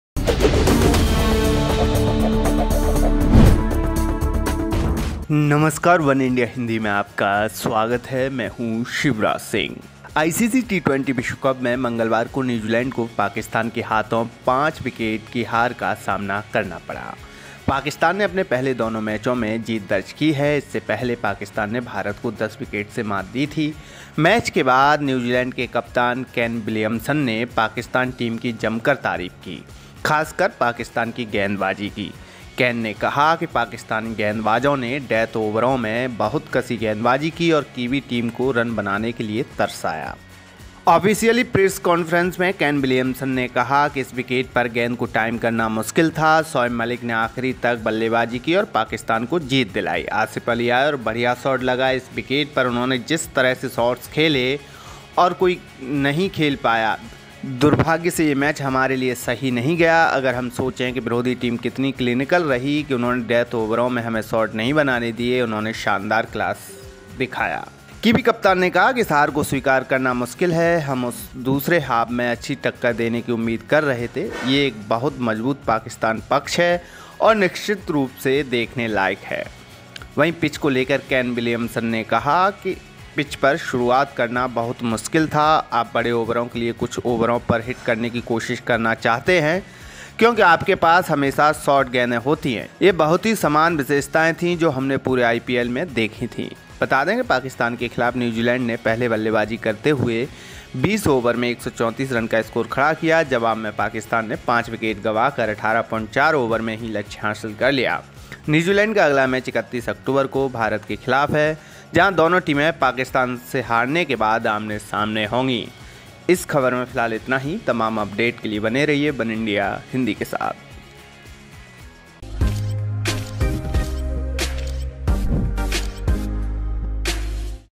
T20 WC NZ Vs PAK: Kane Williamson ने की Pakistan team की तारीफ, देखिए क्या बोले | वनइंडिया हिंदी
New Zealand suffered a five-wicket defeat at the hands of Pakistan in the ICC T20 World Cup on Tuesday. Pakistan have won their first two matches. Earlier, Pakistan had defeated India by 10 wickets. New Zealand captain Kane Williamson has praised the Pakistan team after the match. Especially the bowling of Pakistan.

आईसीसी टी20 विश्वकप (ICC T20 World cup) में मंगलवार को न्यूजीलैंड (New Zealand) को पाकिस्तान (Pakistan) के हाथों 5 विकेट से हार का सामना करना पड़ा. पाकिस्तान ने अपने पहले दोनों मैचों में जीत दर्ज की है. इससे पहले पाकिस्तान ने भारत को 10 विकेट से हराया था. मैच के बाद न्यूजीलैंड के कप्तान केन विलियम्सन (Kane Williamson) ने पाकिस्तान टीम की जमकर तारीफ की है. खासकर पाकिस्तान की गेंदबाजी की.